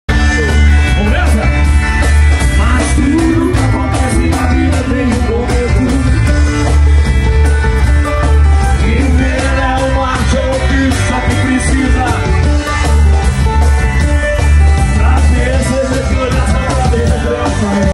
Duo no Violão - Duas Pessoas tocando o mesmo Violão
Este é um Show ao vivo da na cidade de Cristiano Otoni. O vocalista Tales Guesdes e eu no violão fizemos um duo louco do famoso solo do Guns … os dois tocando o violão ao mesmo Tempo!

A galera foi a loucura 🤩🤩🎶